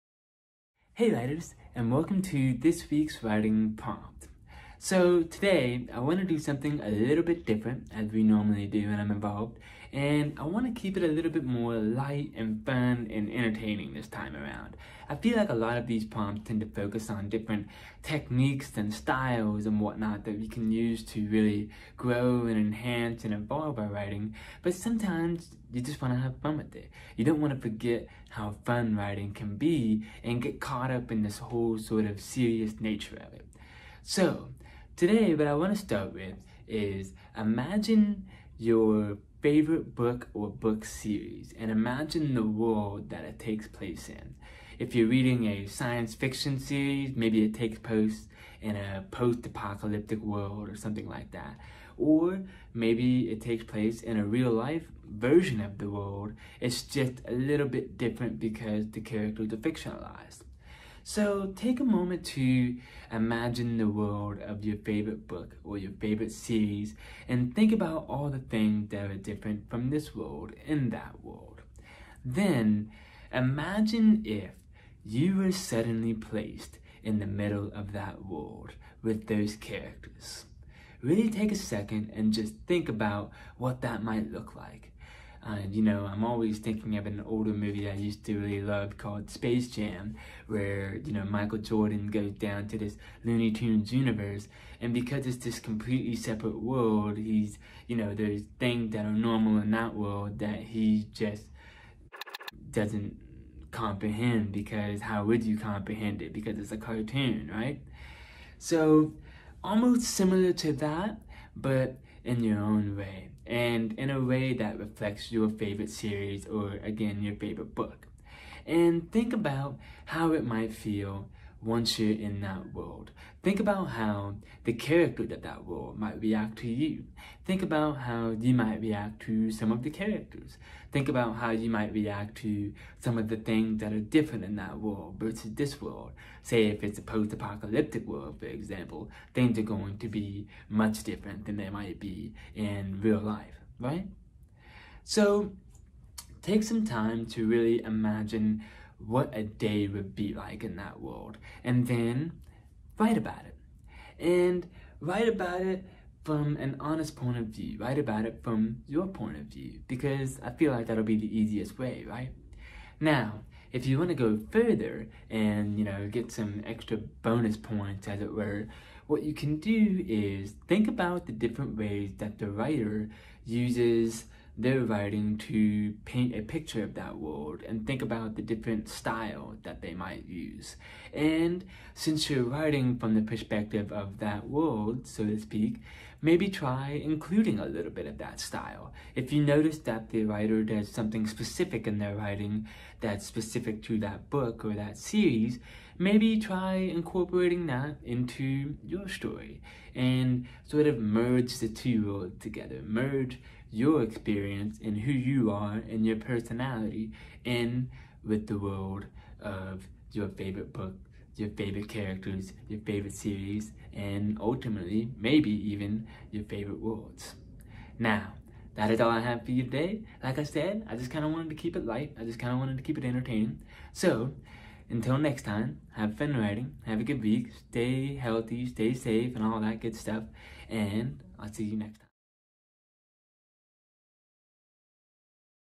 Teen Writers - Exploring Other Worlds
Imagine you could visit the world of your favorite book or book series. What would that be like? Watch and write along with Kody as we explore worlds of all kinds.